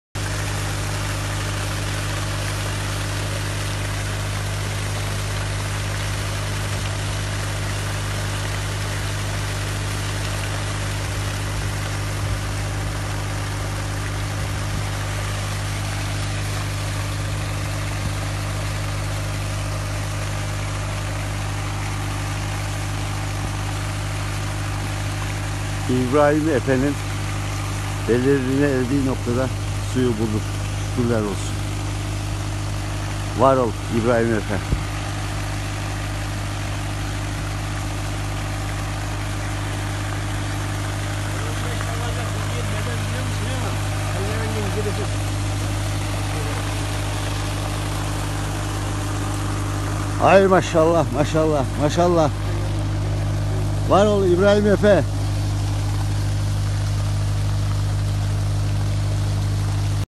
SU ÖLÇÜMÜ, SU TESPİTİ, JEOFİZİK SU ETÜDÜ, ÇUBUKLA SU ARAMA - İZMİR - SELÇUK
İZMİR SELÇUK BELEVİ de Abdullah beyin arazisinde JEOFİZİK SU ETÜDÜ ndeyiz. Evren jeofizik hizm ve tekn Ltd şti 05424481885